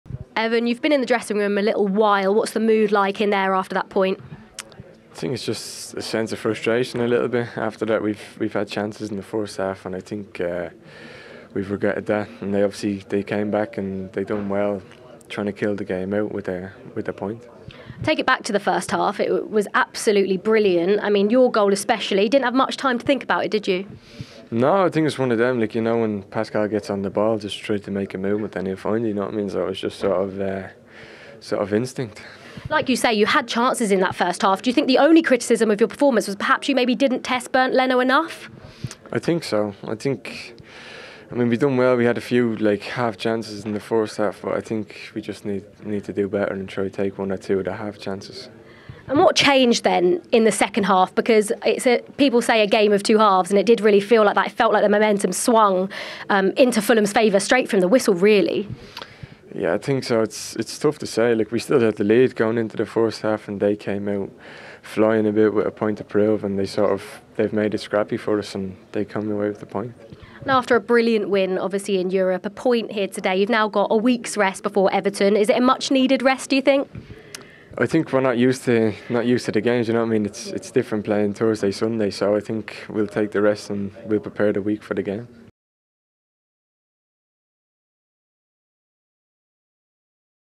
Ferguson On Instinctive Fulham Finish
Evan Ferguson was on target again in Brighton's 1-1 draw with Fulham.